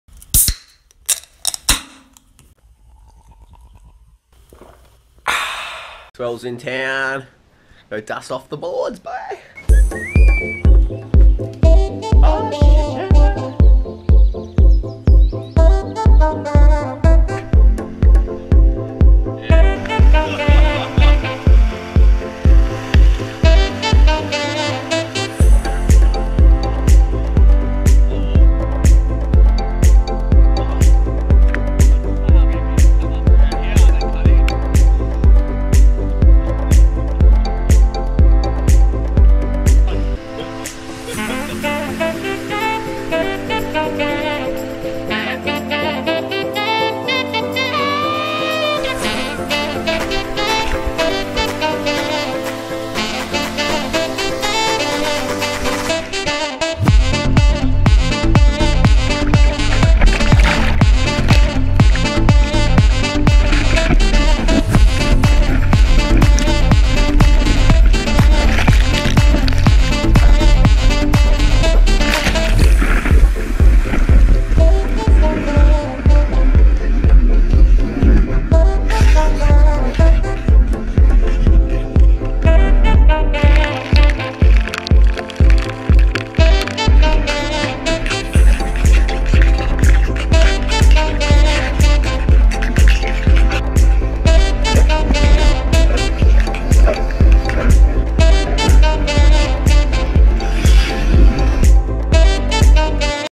Broome CYLONE SURF!!
Before and after cyclones in the north we get some good swell come through! sometimes we have to battle the elements to get a wave!